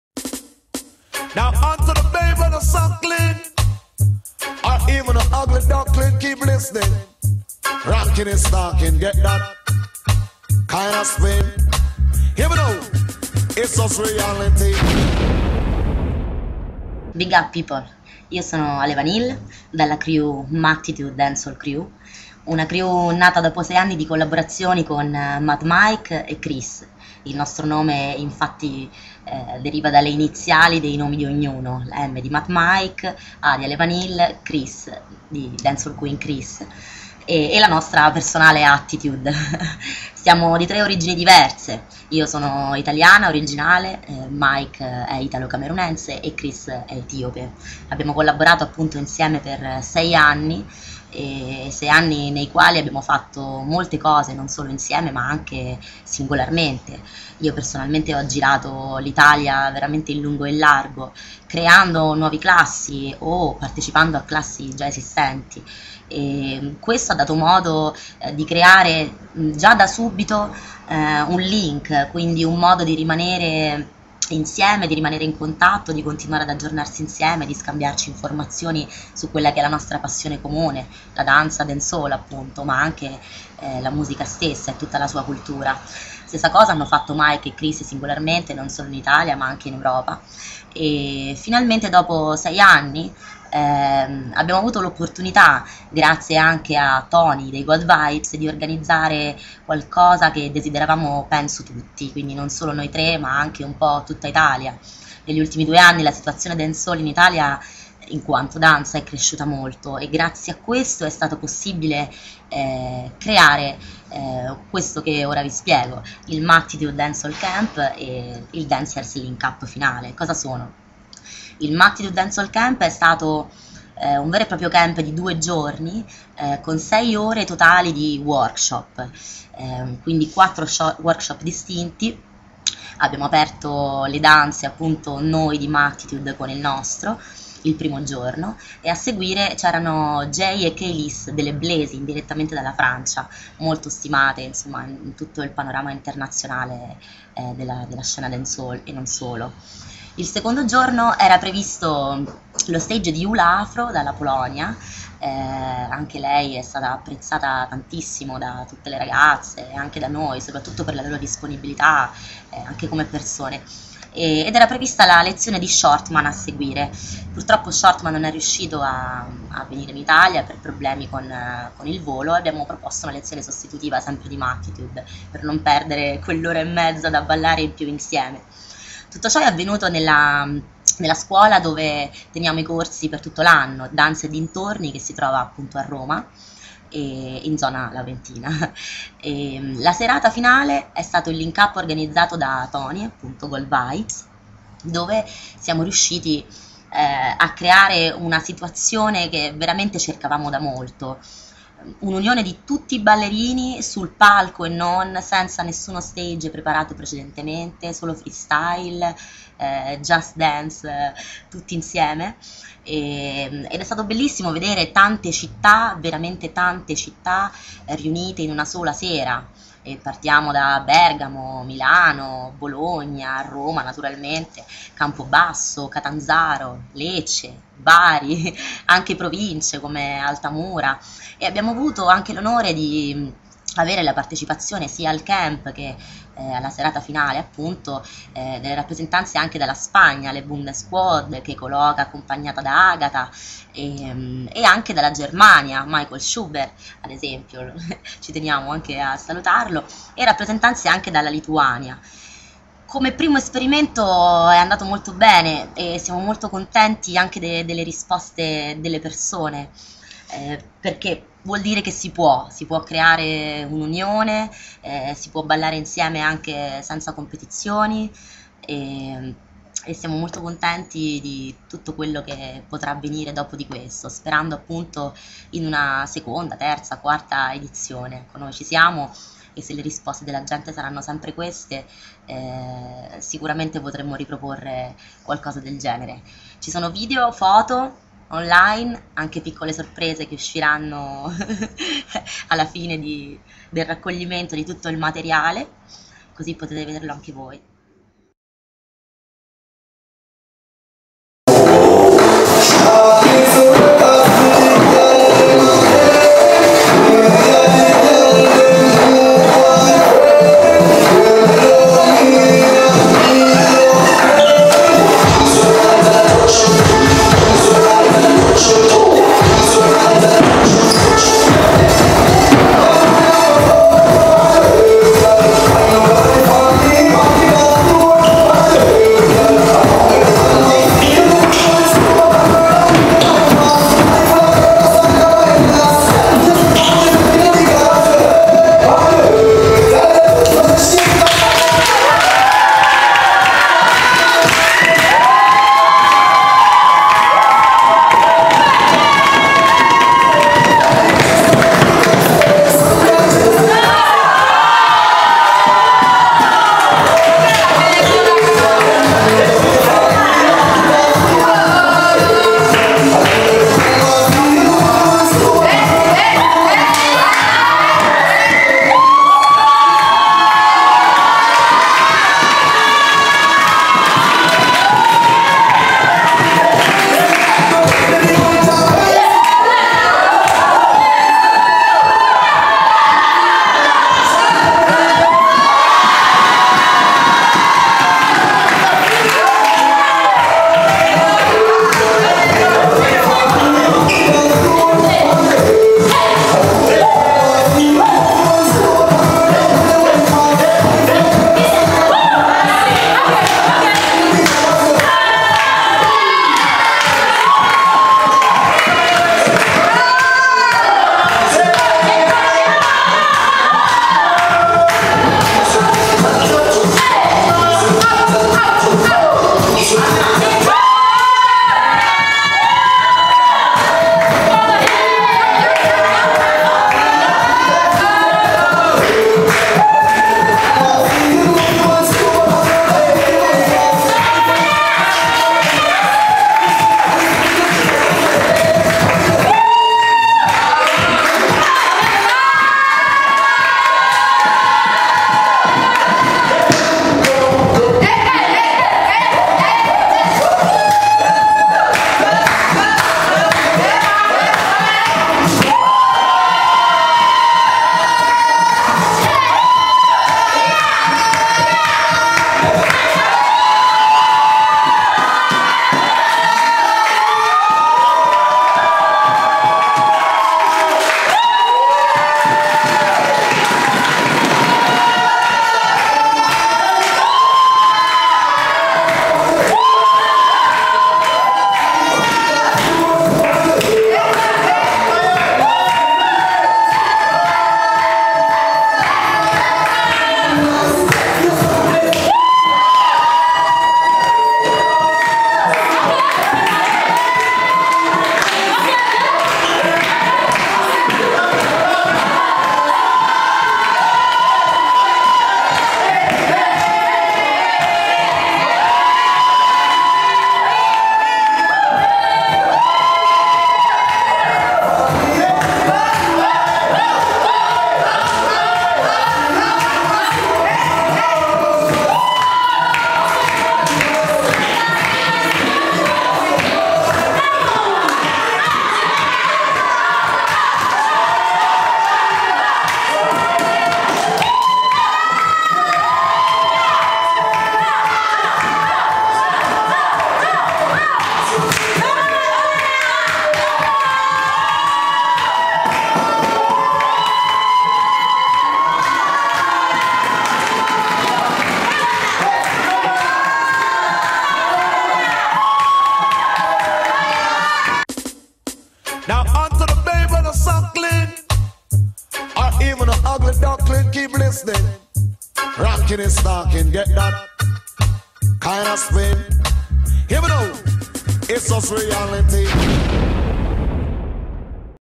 Roma MACTITUDE 1°DANCEHALL CAMP 3-4 Ottobre 2012 ►►►GiornaleArmonia.it
Metti un "mi piace"
Giornale Armonia
Registrato al Tribunale di Taranto numero 638 del 23/11/2004

DANCERS FROM ALL OVER EUROPE:
MAD MIKE, ALEVANILLE & CHRISS
AKA MACTITUDE DHCREW (ITALY)

SPECIAL REGARDS:
CLOTHING BY ETHIOPIA MI SEH...